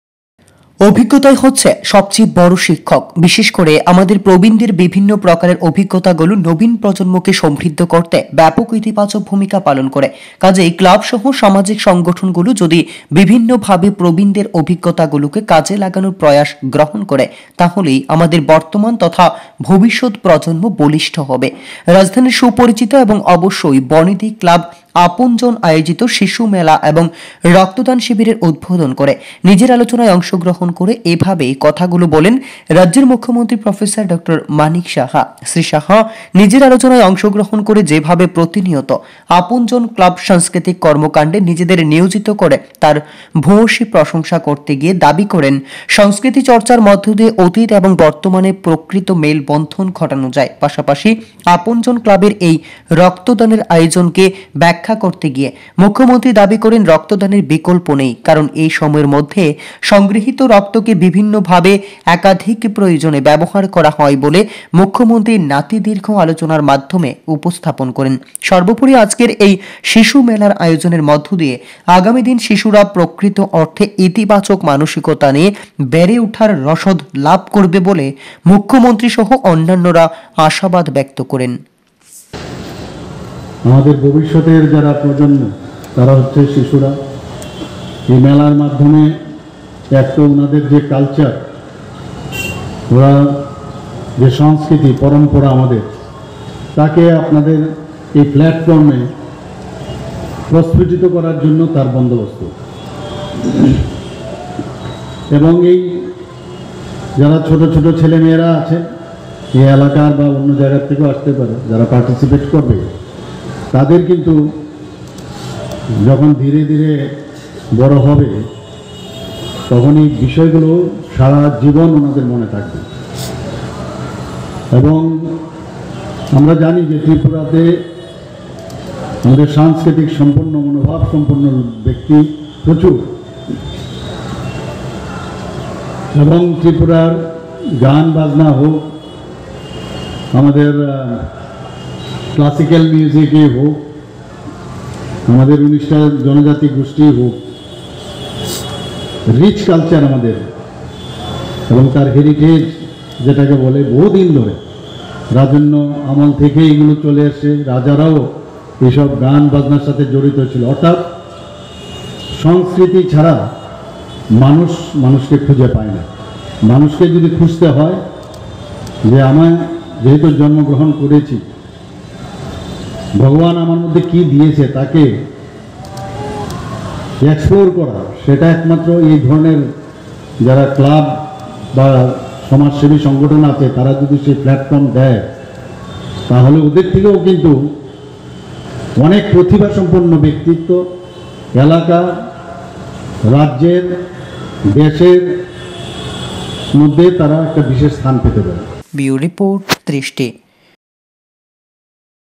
আপনজন ক্লাবে আয়োজিত শিশু উৎসব ও রক্তদান শিবির।।
রবিবার রাজধানীর সুপরিচিত বনেদি ক্লাবে আয়োজিত শিশু মেলা এবং রক্তদান শিবিরের উদ্বোধন করেন রাজ্যের মুখ্যমন্ত্রী অধ্যাপক ডঃ মানিক সাহা।।